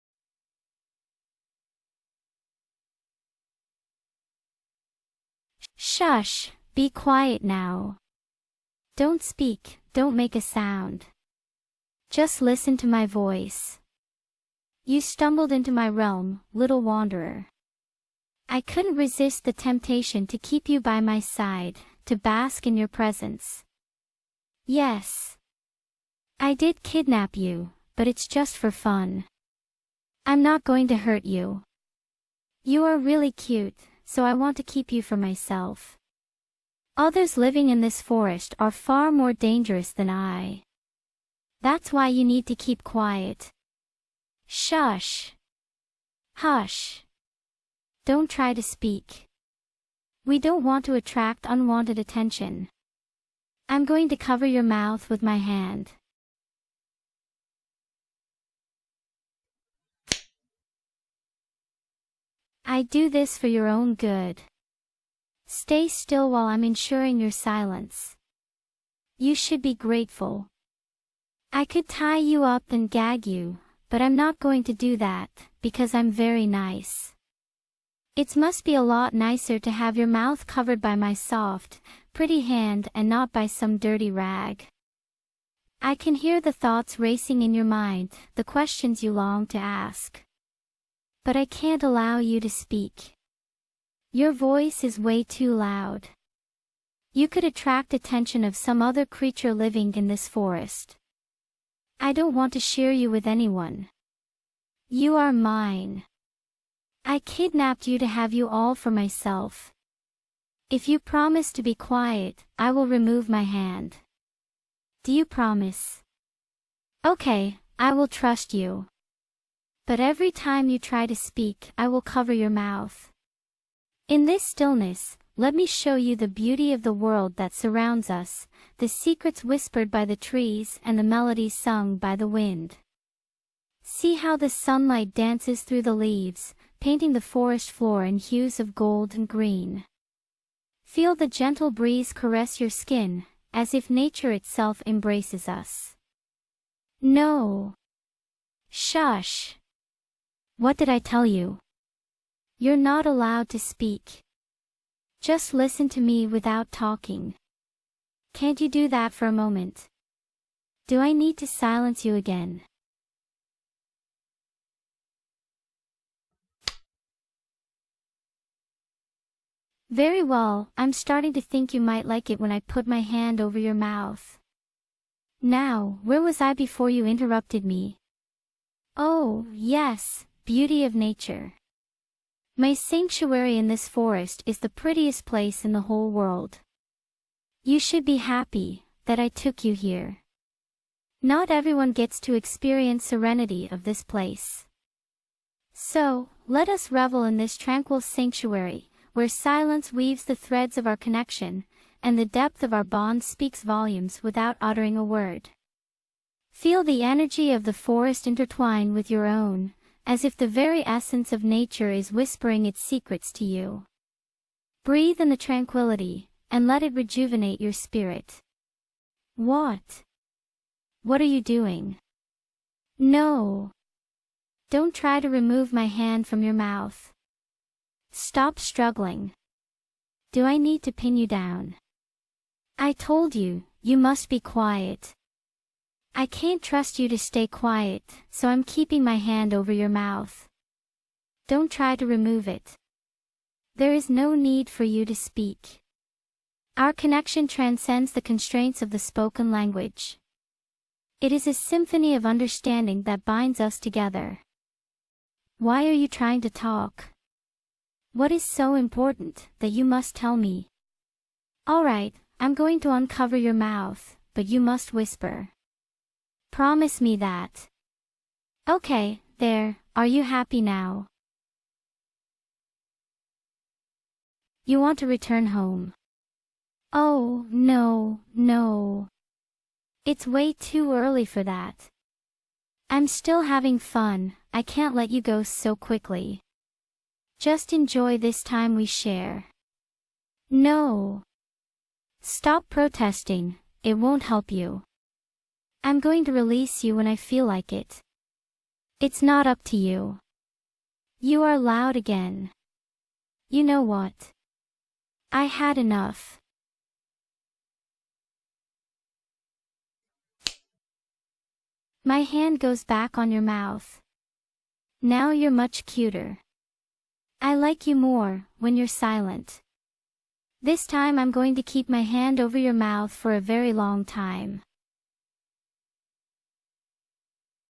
ASMR - Fox Girl kidnaps you and keeps covering your mouth [f4a] [covering mouth] [kidnapping]
While wandering in the forest you got kidnapped by Fox Girl. She covers your mouth with her hand to ensure your silence. Allow yourself to embrace the beauty of silence and discover the profound depth of connection it can bring. Relax, close your eyes, and let the serenity of this ASMR audio wash over you.